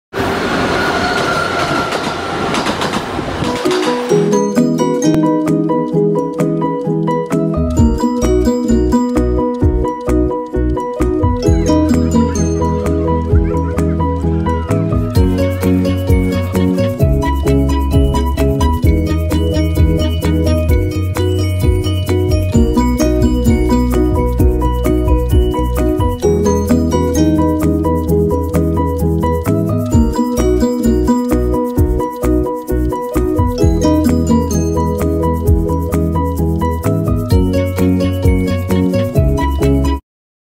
Pingxi line | eatseetravel.net
A day on Pingxi railway, visiting Pingxi, Shifen and Shifen waterfalls